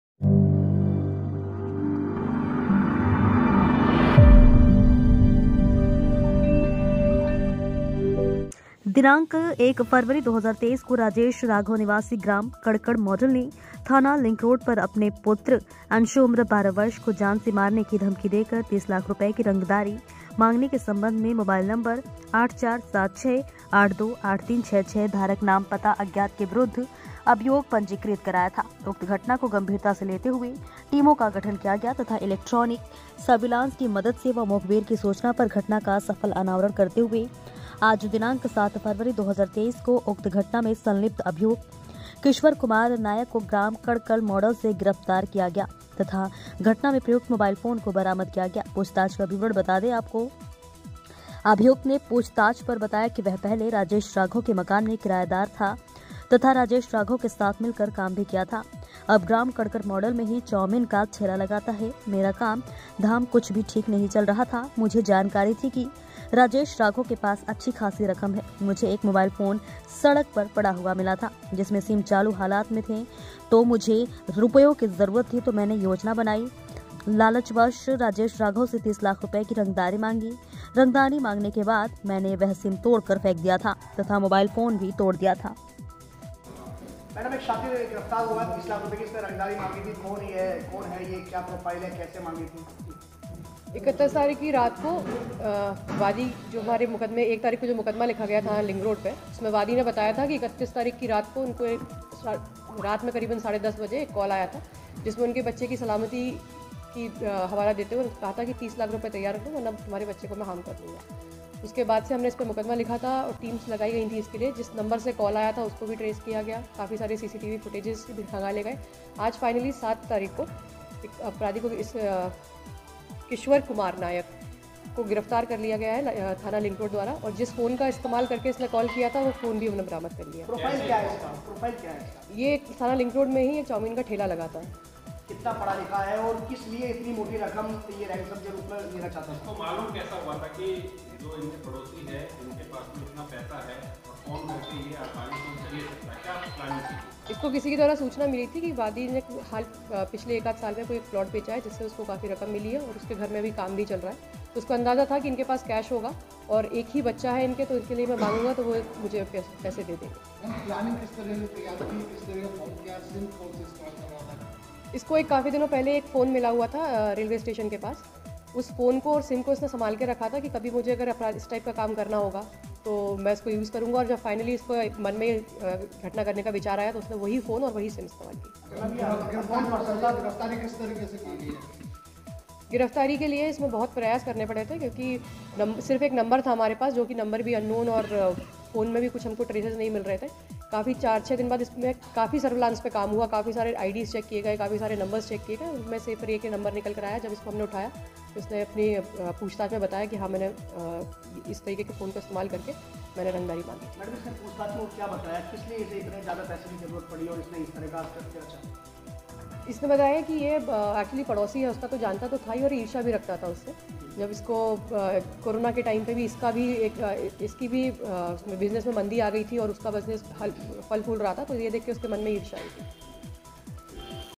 थाना लिंकरोड पुलिस द्वारा 30 लाख रुपये की रंगदारी माँगने वाले अभियुक्त को किया गिरफ्तार
*दिनांक 1/2/23 को  राजेश राघव नि0 ग्राम कड़कड़ माडल ने थाना लिंकरोड पर अपने पुत्र अंशु  उम्र 12 वर्ष को जान से मारने की धमकी देकर 30 लाख रूपये की रंगदारी मांगने के सम्बन्ध में मोबाइल* नं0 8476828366 *धारक नाम पता अज्ञात के विरुद्व अभियोग पंजीकृत कराया था । उक्त घटना को गंभीरता से लेते हुए टीमों का गठन किया गया तथा इलैक्ट्रोनिक सर्विलांस की मदद व मुखविर की सूचना पर घटना का सफल अनावरण करते हुए आज दिनांक 7/2/23 को उक्त घटना में संलिप्त अभियुक्त किश्वर कुमार नायक  को ग्राम कड़कड़ माडल से गिरफ्तार किया गया तथा घटना मे प्रयुक्त मोबाईल फोन को बरामद किया गया*
*पूछताछ का विवरण